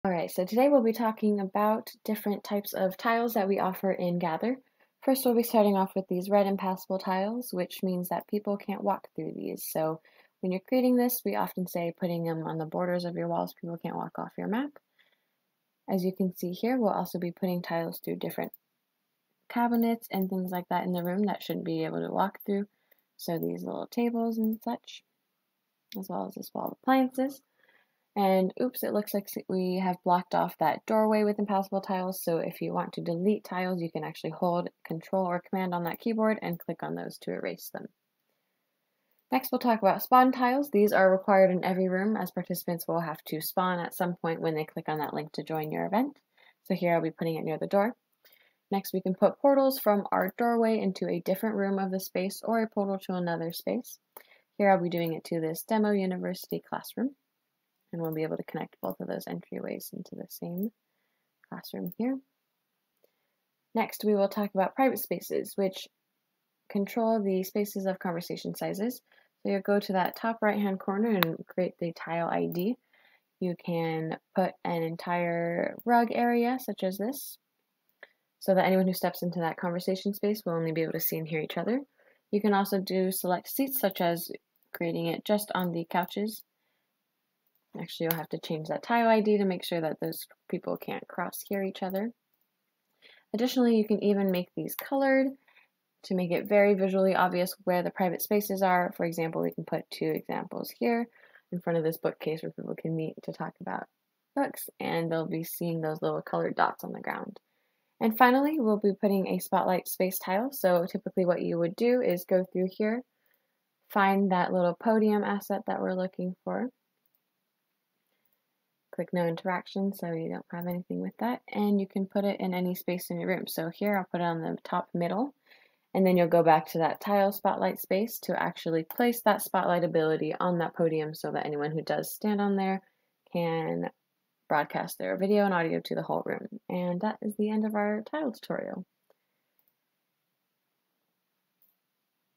[Older UI] Map Maker Types of Tiles
Note: This video was made using an older version of the Mapmaker. Although the interface looks very different, many of the original application functions are the same and the general advice is still relevant today!